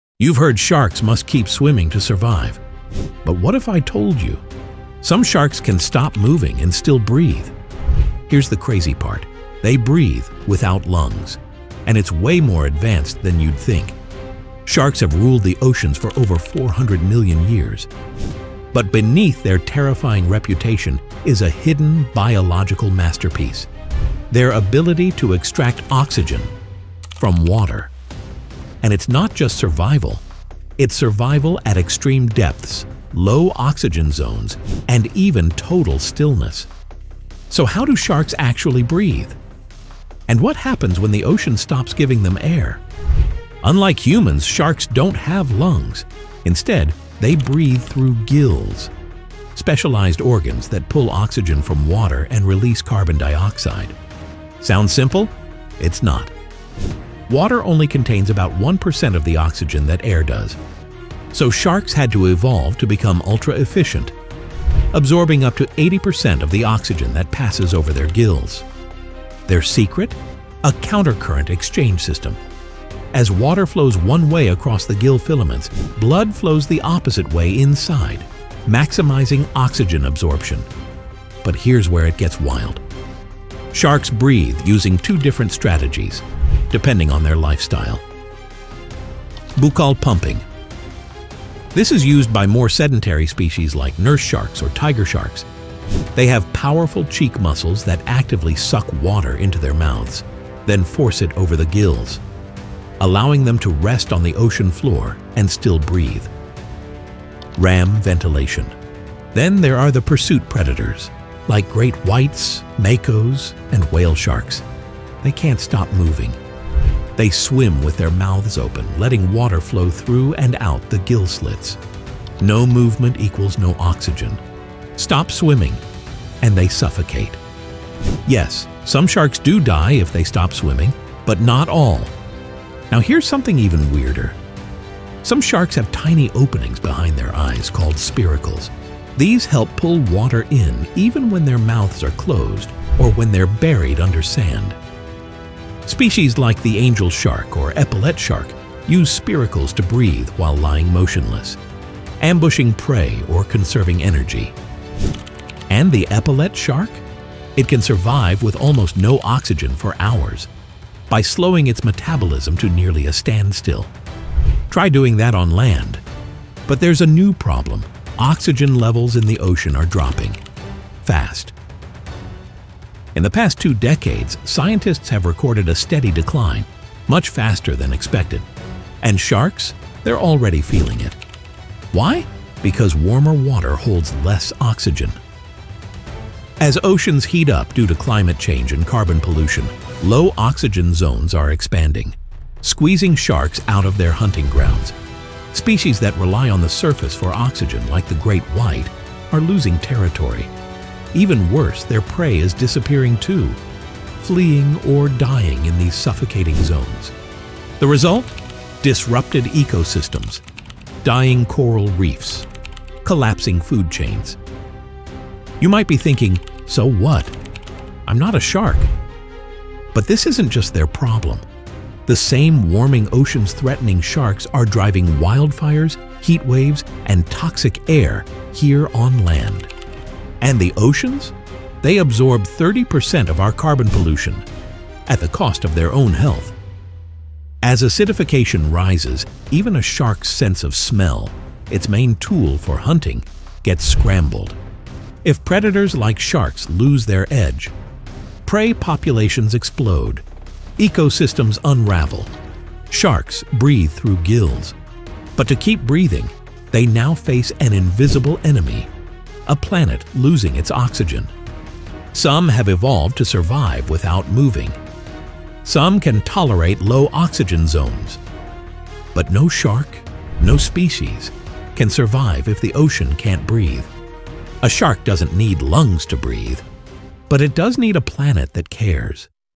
How Do Sharks Breathe? (Can Sharks Drown?)
How do sharks breathe underwater? And can sharks drown? This video breaks down how gills work, and why some sharks must keep swimming to survive. Do sharks drown if they stop swimming? It depends on the species, some will suffocate without movement, while others have evolved unique ways to breathe at rest.

You'll learn the difference between buccal pumping and ram ventilation, and discover how some shark species can actually breathe while resting.
We’ll also explore how spiracles help sharks like the epaulette survive in low-oxygen environments.
And as oxygen levels in our oceans drop, you’ll find out why this ancient breathing system is now under serious threat.

Check out this educating documentary video and learn more facts about the Sharks! ✅